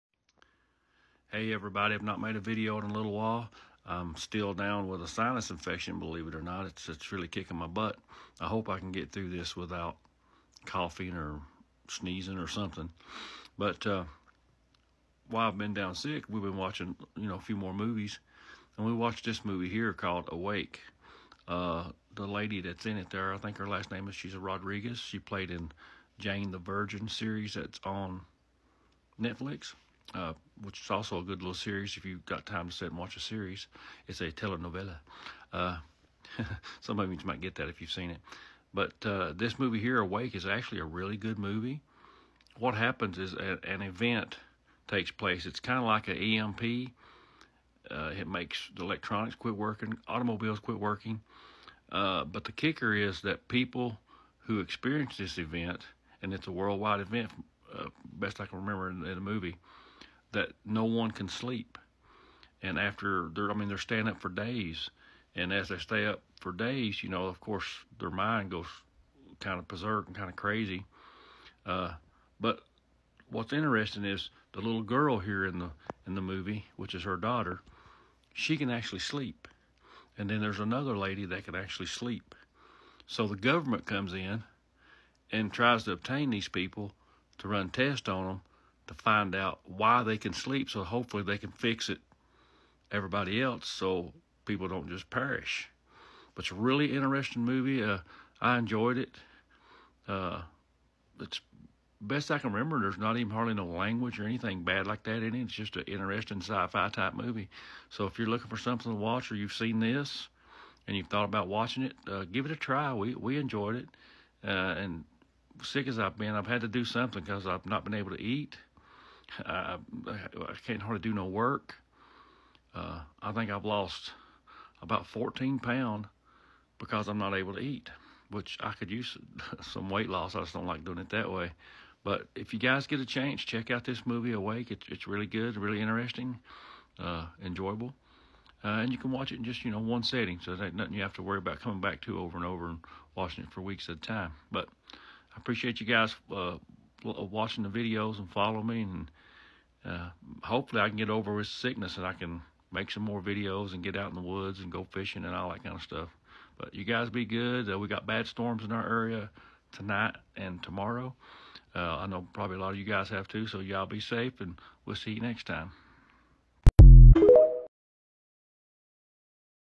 Awake | Movie Review | From My TikTok Channel
I thought I would copy over some of my TikTok videos. While I was sick, I watched several movies and attempted to do some kind of little review of them.
If you are interested in following me on TikTok then jump on over there

Thanks
Thomas